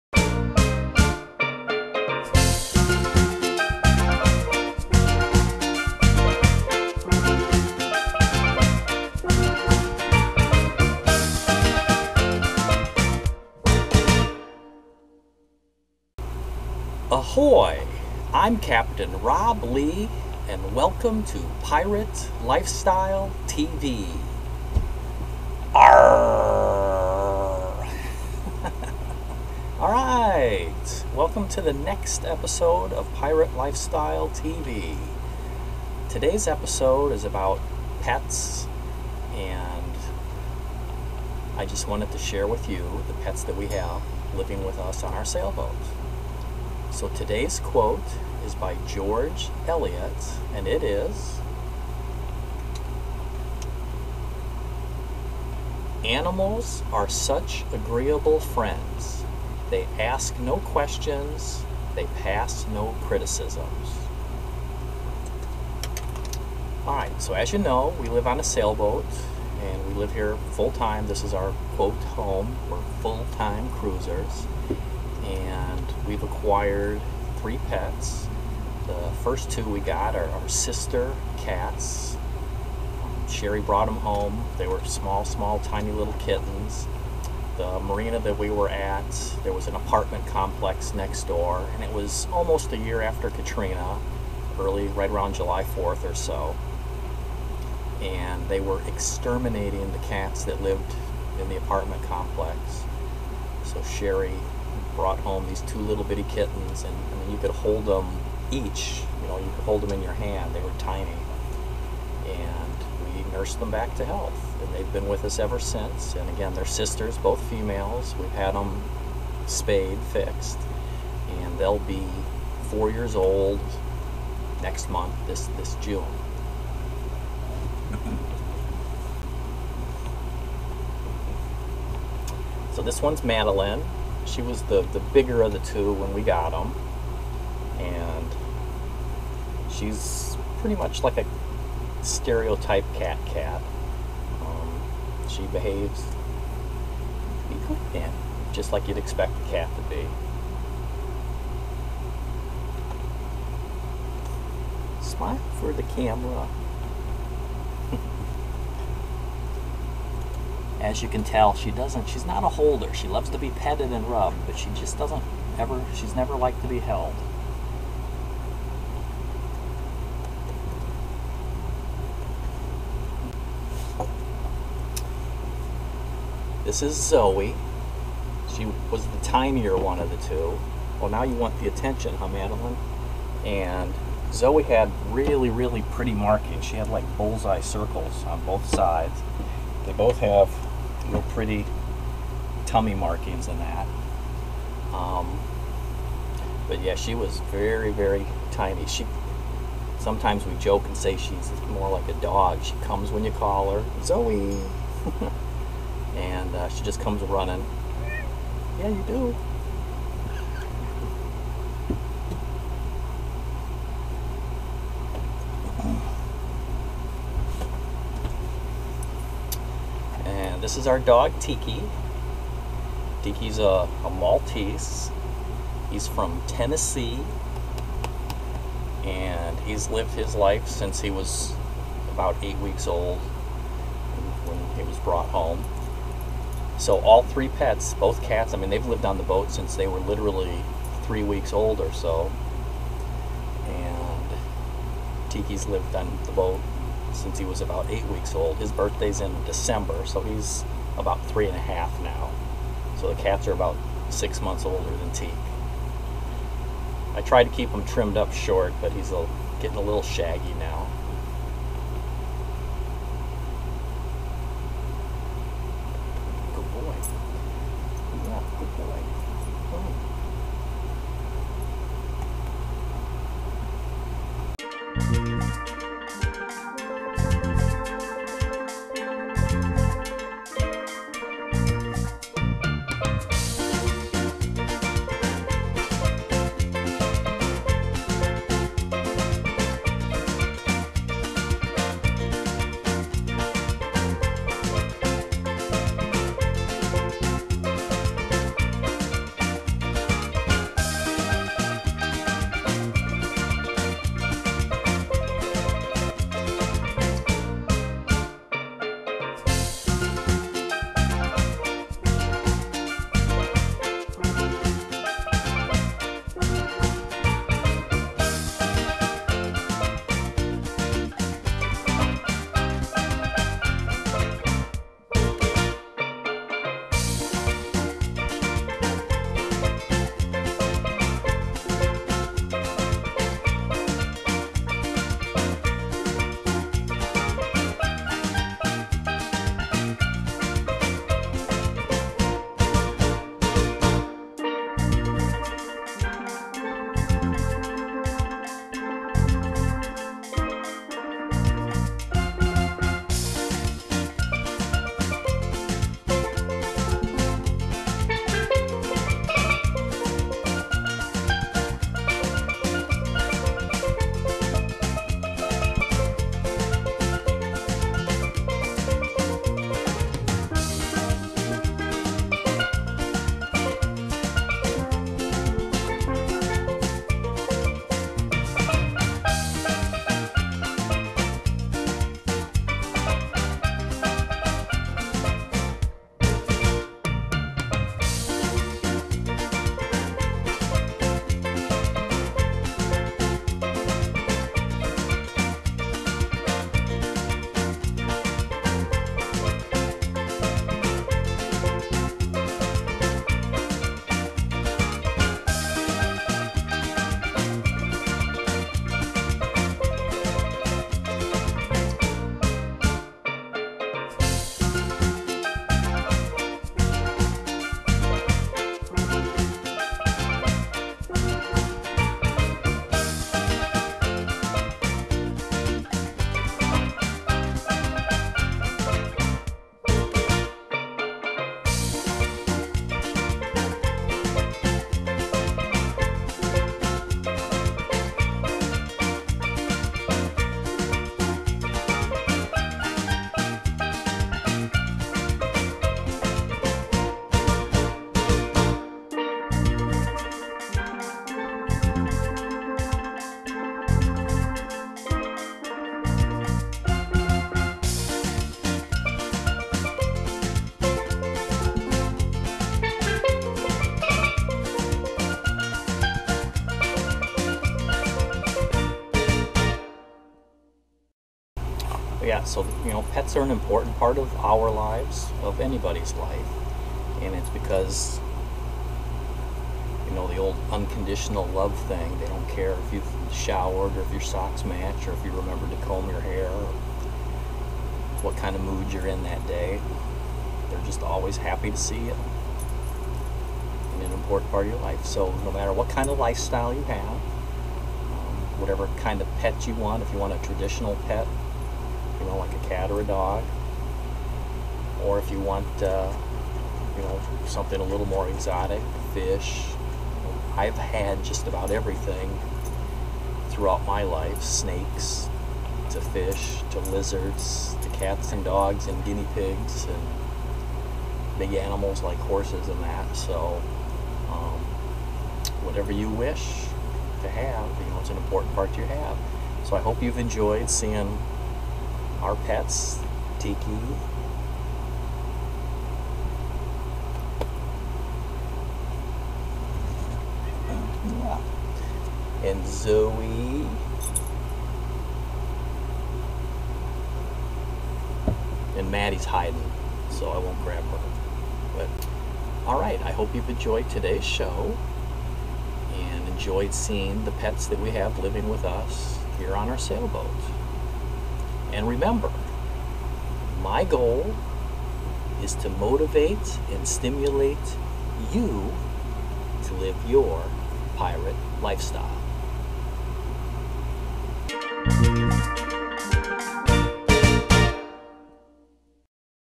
Pirate Lifestyle TV ™ - Episode 015 Pets
Episode 015 for the all NEW Pirate Lifestyle TV Show. Create your own Reality! How do you see the world around you? Pets and how important they are to us. See Tiki, Maddie, and Zoe in action.